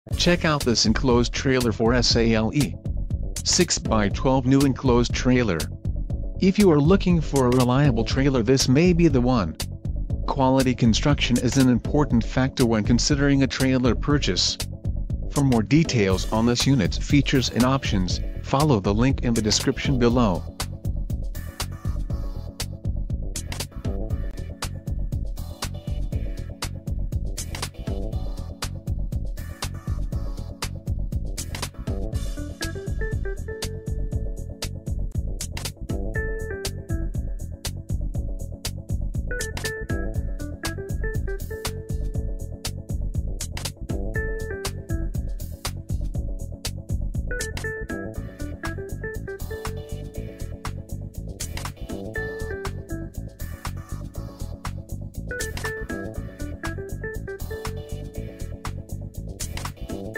ENCLOSED TRAILER for SALE! 6x12 New Enclosed Trailer - S...
ENCLOSED TRAILER for SALE! 6x12 New Enclosed TrailerThis trailer at our Ft Pierce Lot NOW at our low price of ONLY $2086Factory Retail Price of this trailer (factory in GA) is $1834Call us for pricing information as additional fees / taxes may apply to the above prices, depending on where you pick up this trailerFinancing may be available for you on this trailer! OSBnon-powered roof ventWhite .024 aluminum exteriorapproximately 1200lbs. (dry)Looking for something else?